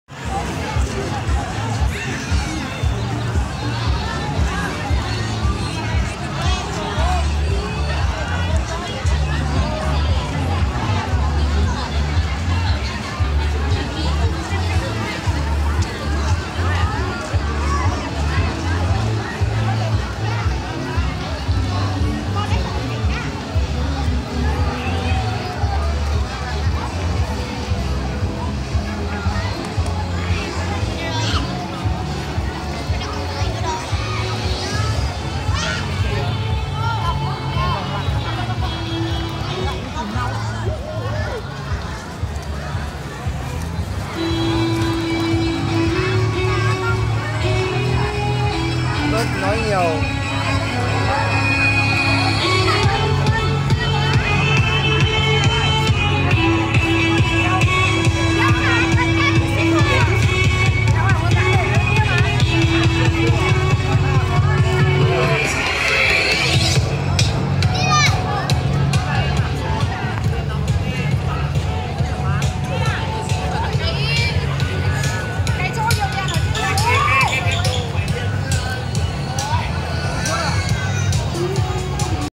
Women's Lao,Dao people ethernic of growup local on the warkstress ,Tan Uyen distris,Vietnam country.
Women's Lao,Dao people ethernic of growup local on the warkstress ,Tan Uyen distris,Vietnam country. Women's Lao,Dao people on the warkstress of Tan Uyen distris,Vietnam country